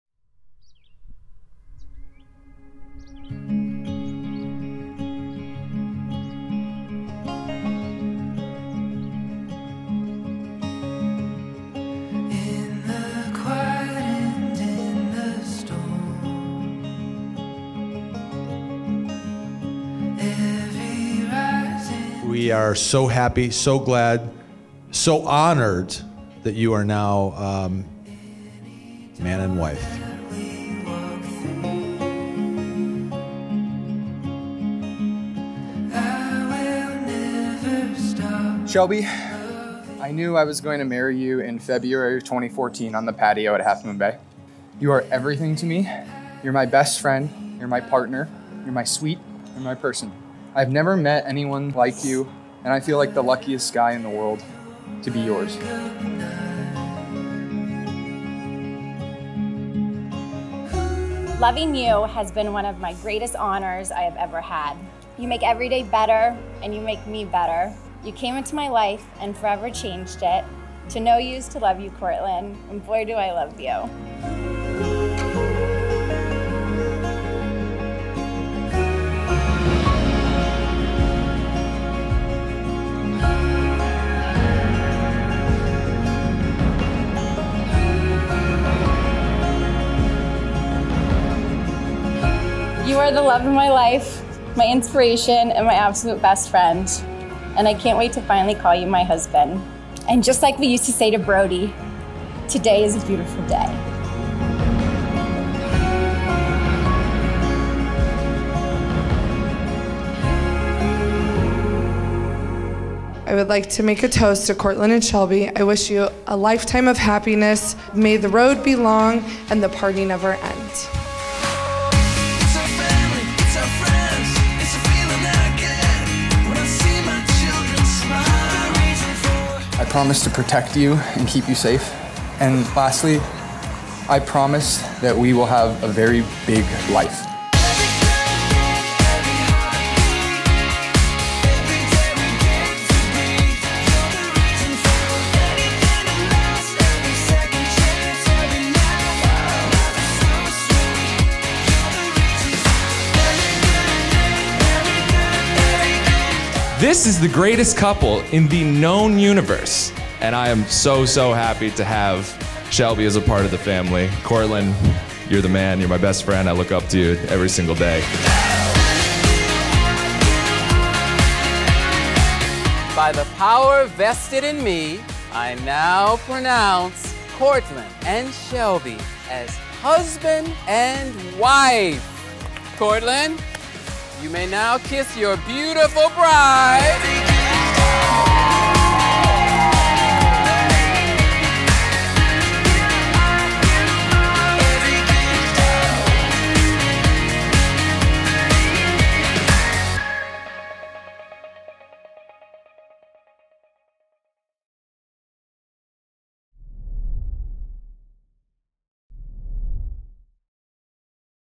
Shelby and Courtland's Fairy-Lightshow Wine Cellar Wedding Video from St Helena, California
Shelby and Courtland's wedding was the picture of charm! Married under the fairy lights, sharing a kiss in groves of white roses and purple lavender, and then dancing the night away under garlands of cypress and a colorful rave of glowsticks—these two had a night to remember!

Wedding Venue: The Culinary Institute of America, St Helena, California
Wedding Photography: Willow and Ben Photography
Wedding Planning: Stratford & Pearl
Wedding Sound: Cruz Entertainment